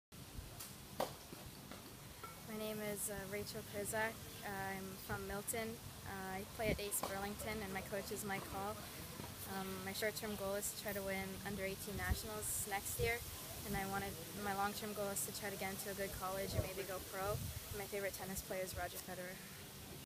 Rachel Krzyzak 2017 U18 Girls Singles Champion
Rachel shares her goals with MTC